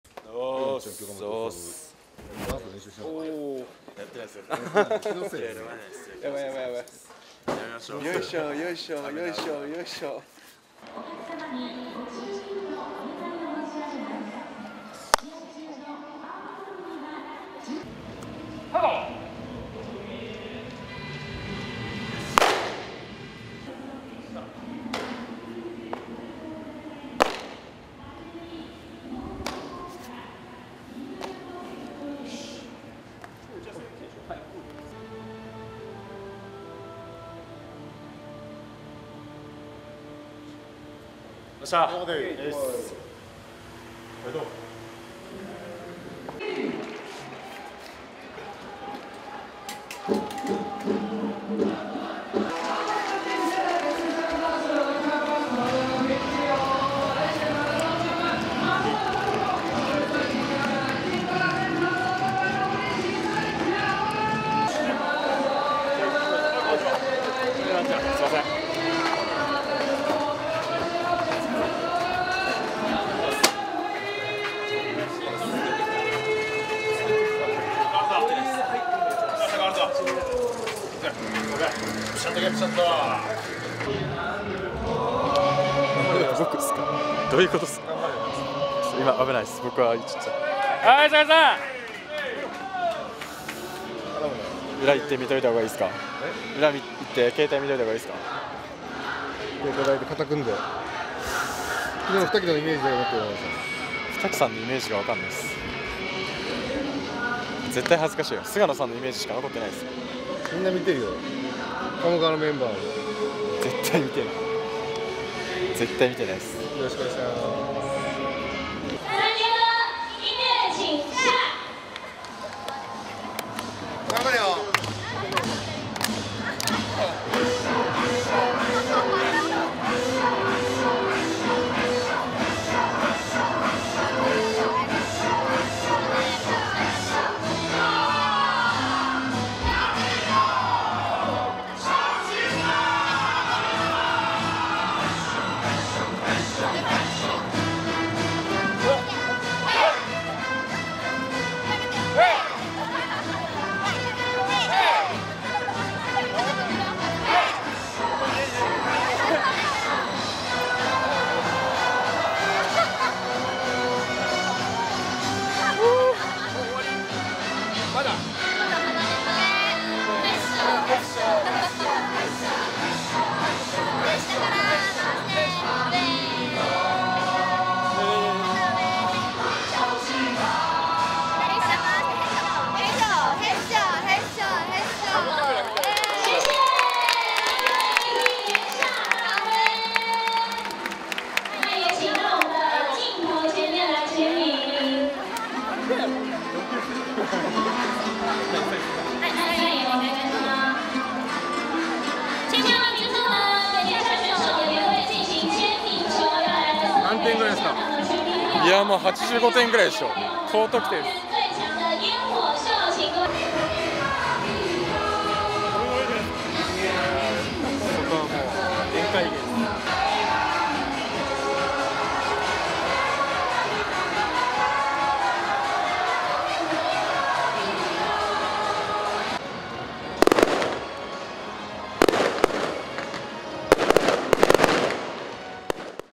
岩下の勝利のダンスにカメラが接近！【広報カメラ】
2018年の台湾遠征初戦に先発し5回4安打無失点の好投を披露した岩下投手のダンスにカメラが接近しました！